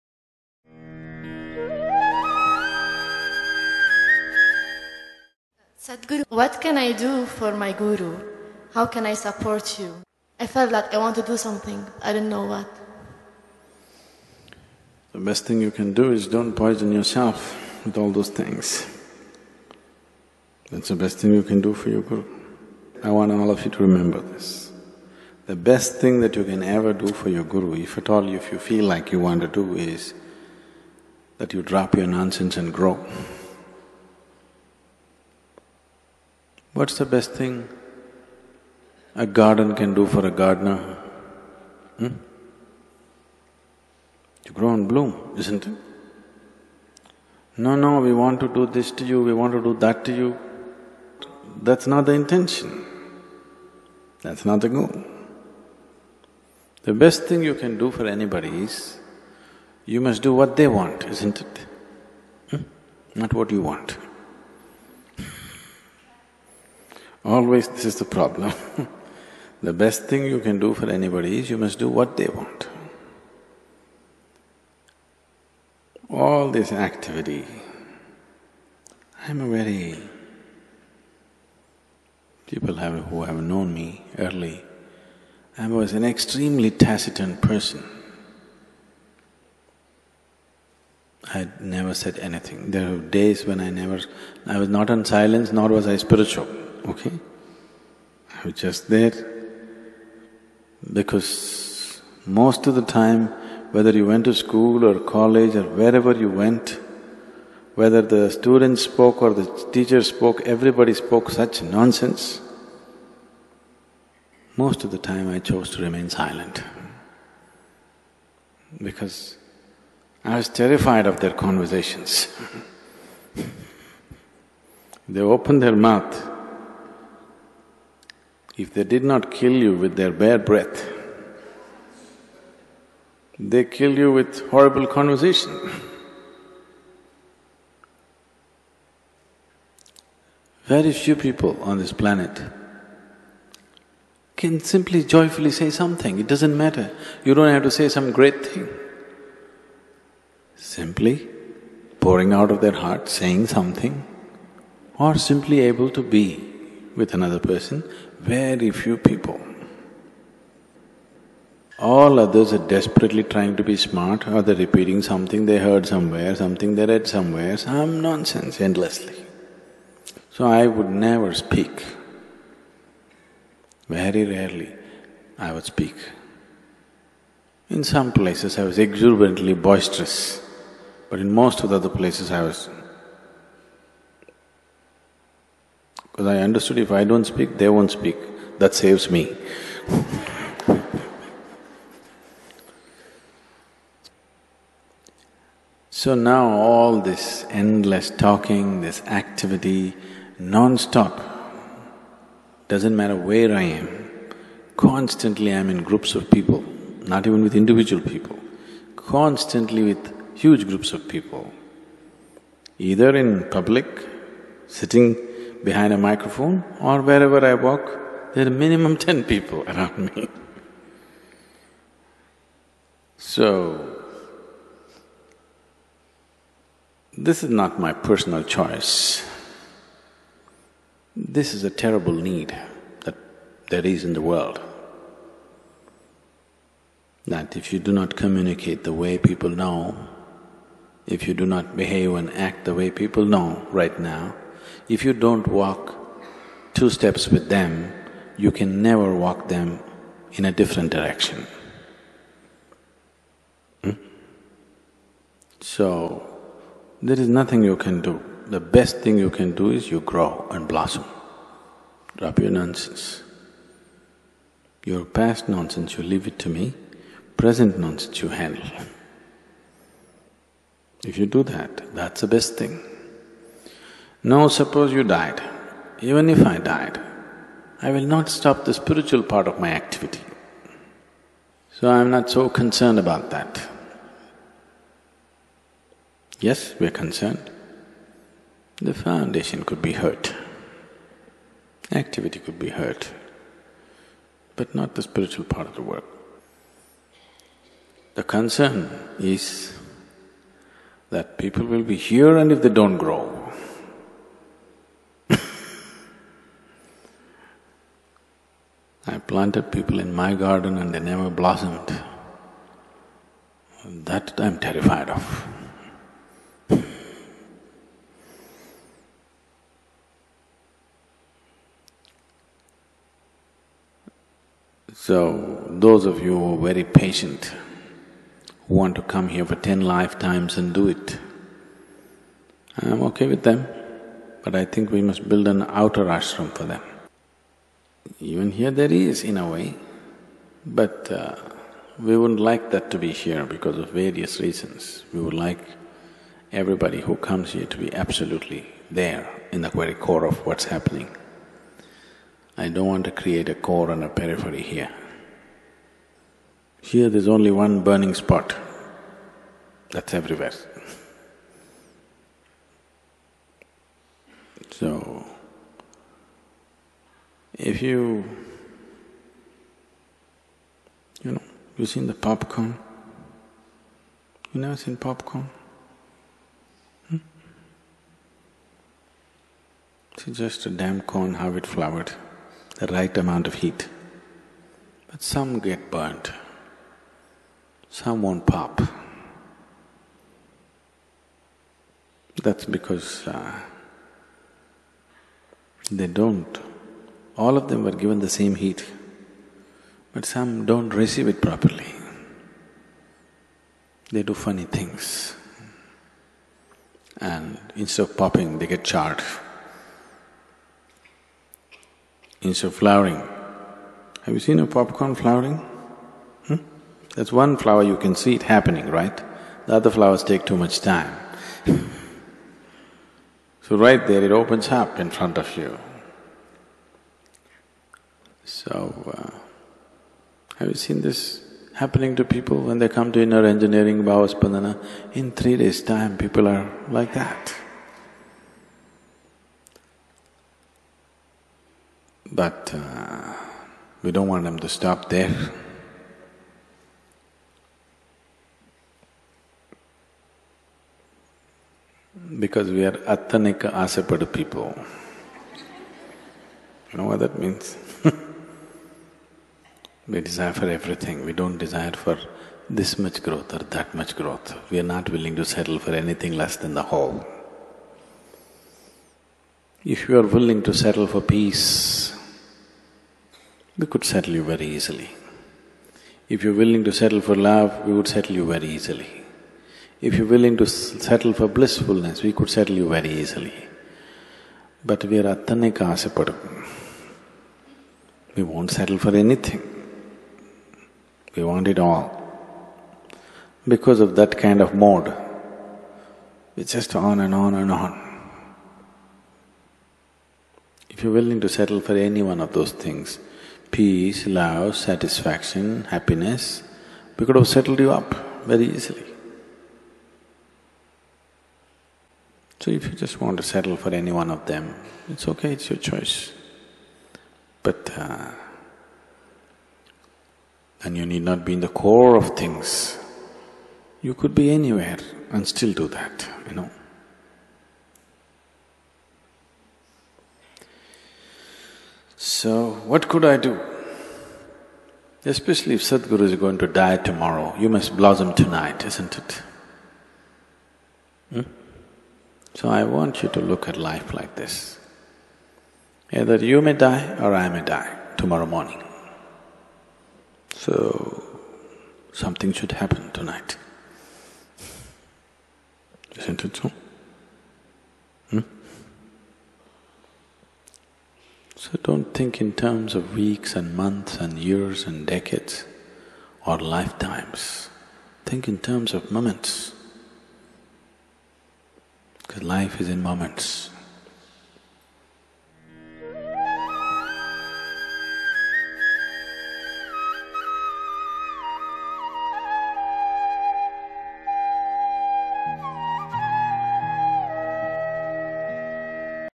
你能为古鲁做的最好的事是什么？| Sadhguru 萨古鲁
萨古鲁说，你能为你的古鲁做的最好的事，就是放下你的废话，成长。 一个花园能为园丁做的最好的事情是什么? 是成长和绽放。